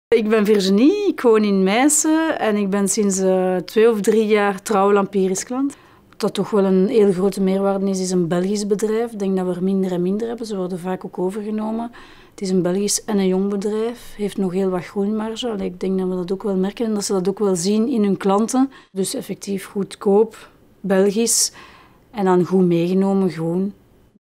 Lampiris interviews | Virginie de Klippel (Brussel)
| Maak kennis met Virginie, tevreden klant bij de 100% Belgische, groene en goedkopere energieleverancier Lampiris